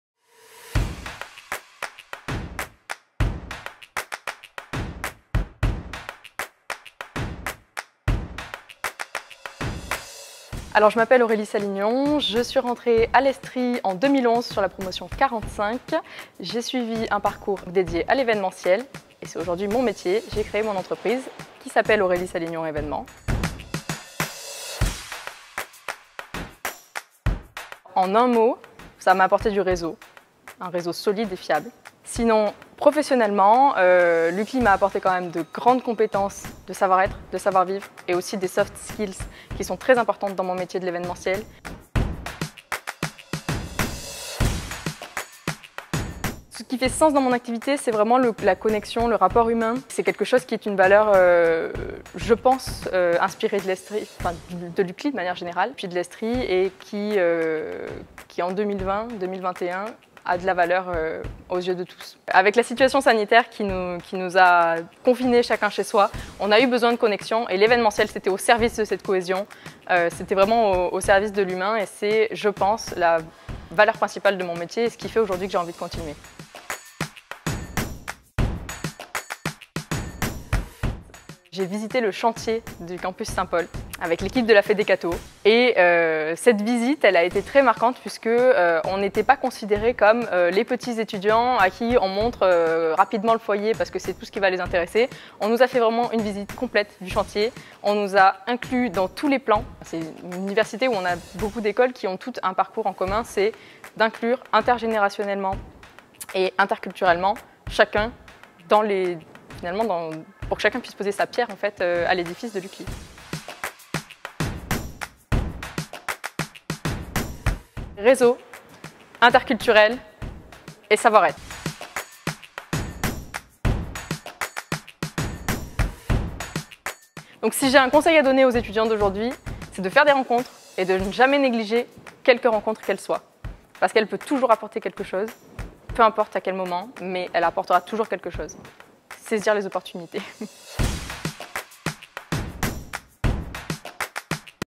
Témoignage d'alumni | Aurélie Salignon ESTRI 2017
Ancienne élève de l’ESTRI, Aurélie Salignon a trouvé sa voie dans l'événementiel en montant sa propre boîte Aurélie Salignon Événements !

Un des objectifs principaux de l’UCLy est d’accompagner les étudiants dans leur insertion professionnelle. Réseau, compétences, rapports humains, elle nous raconte en vidéo ce que son parcours à l'Université Catholique de Lyon lui a apporté.